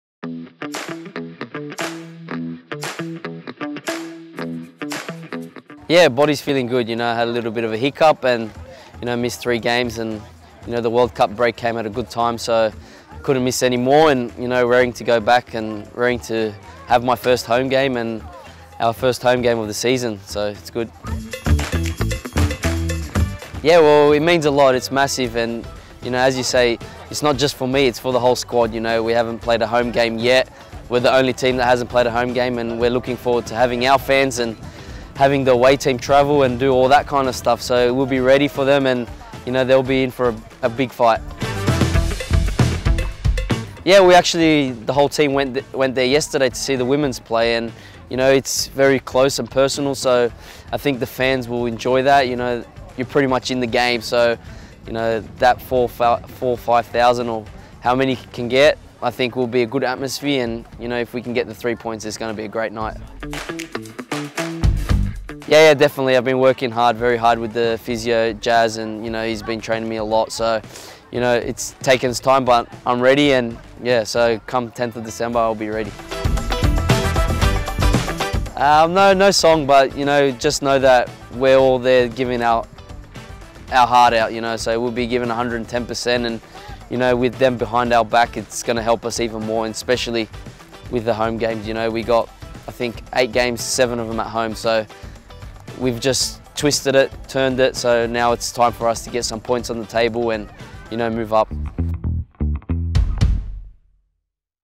Perth Glory I Mustafa Amini I Western United Match Preview I Back at home
We're finally playing back in front of our Members and Fans at our new (temporary) home, Macedonia Park and the lads are pumped